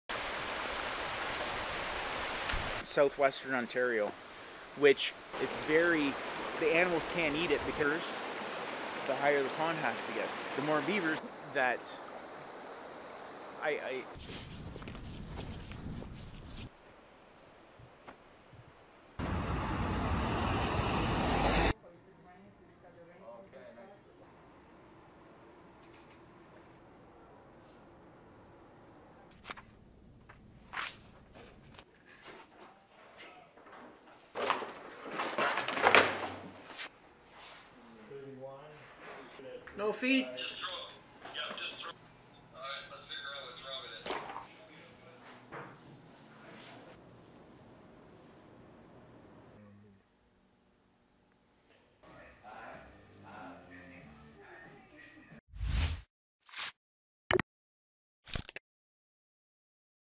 Canadian trappers wrestle with loss of Russia, Ukraine fur markets
Canada is the world's largest producer of wild furs, but few manage to make a living anymore solely in the fur trade, which dates back 400 years to the first Indigenous trades of pelts to European explorers. Now, Canada's oldest industry will have to cope with the lack of access to markets in Ukraine and Russia -- the latter being the world's second-largest for fur but facing Canadian and allied economic sanctions.